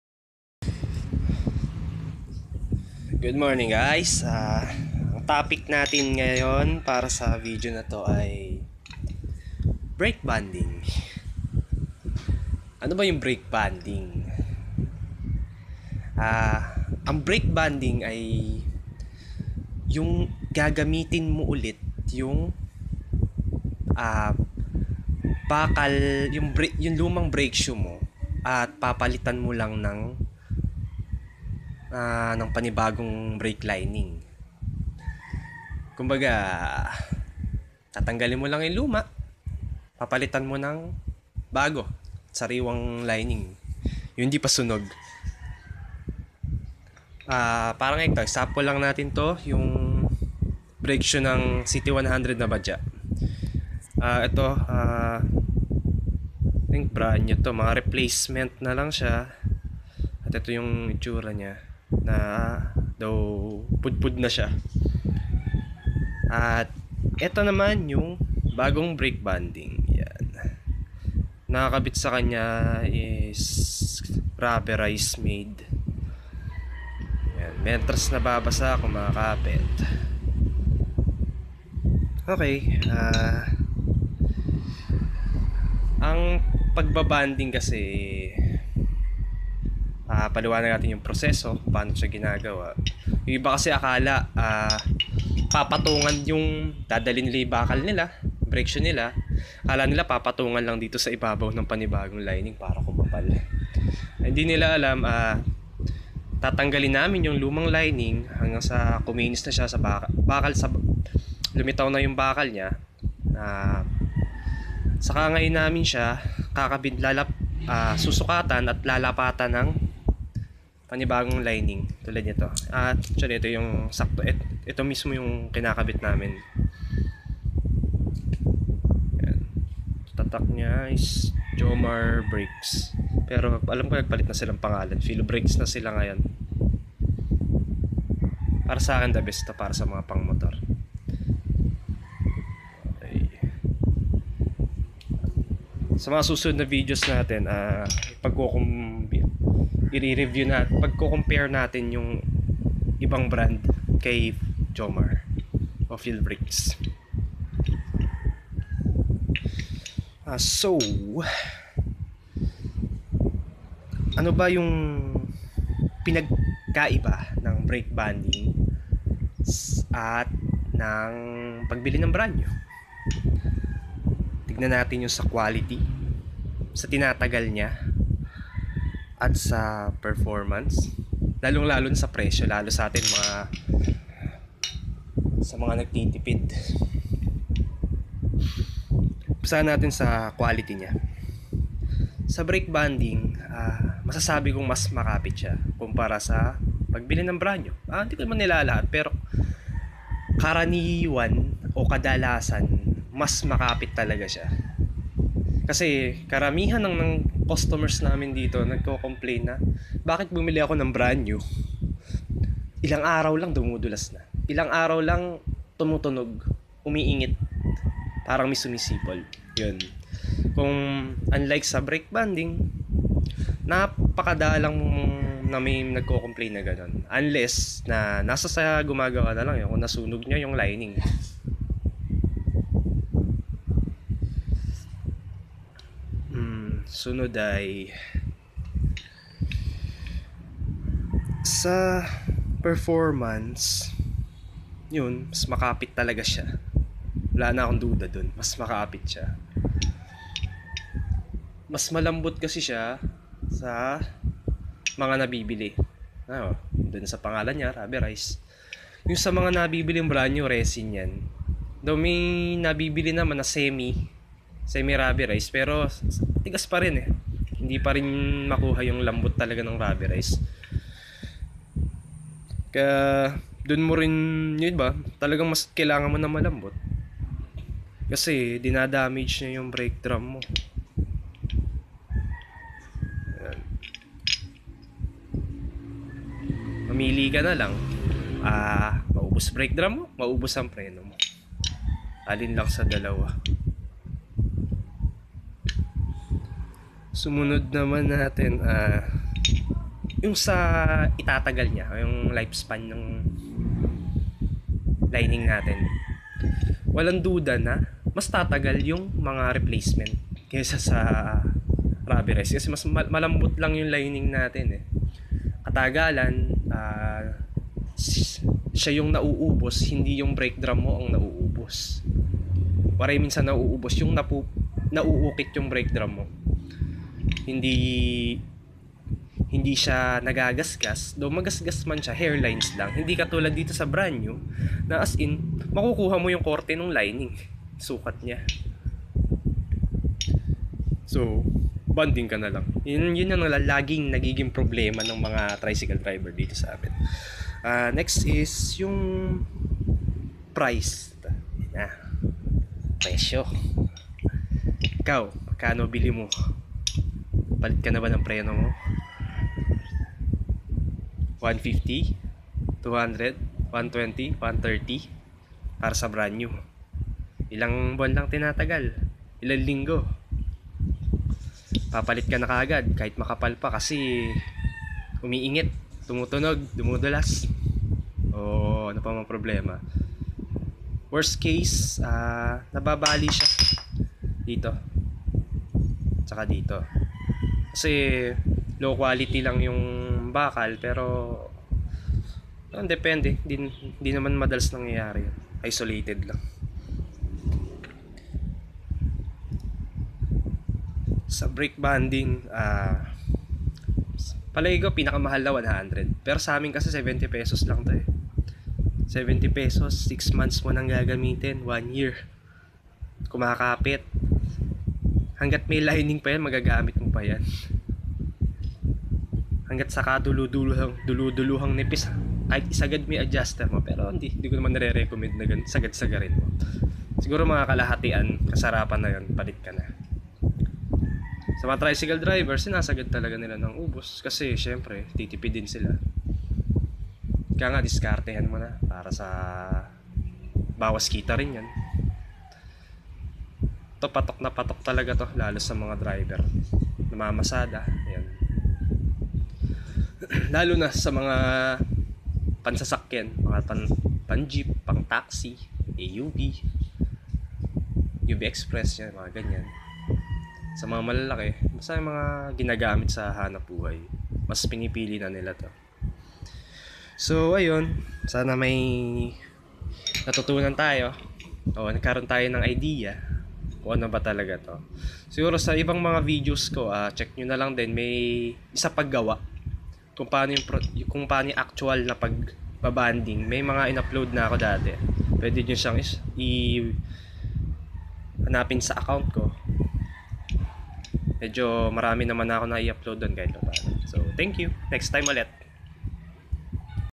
Brand new brake shoe o brake rebonding ?
sa taon na iupload ko ang video na ito ay 2019 yan ang quality ng mga linning,ngbabago po ang kalidad ng linning bawat taon dahil ngtataas din ang presyo ng materyales kaya binabago din ng factory ang timpla ng linning nila, maari na sa susunod na taon ay ibang brand na ginagamit ko dahil sa ngbago na ang kalidad ng linning sa panahon o taon iyon,  gagawa po ulit ako ng same video sa 2021